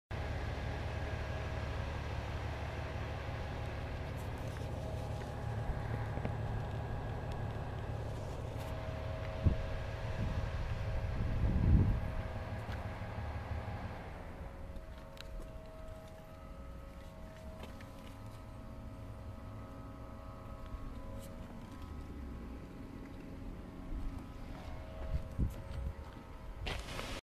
GovDeals: 2005 Epoke Tow Behind Spreader
2005 Epoke Tow Behind Spreader sold by Ingersoll, ON on GovDeals.com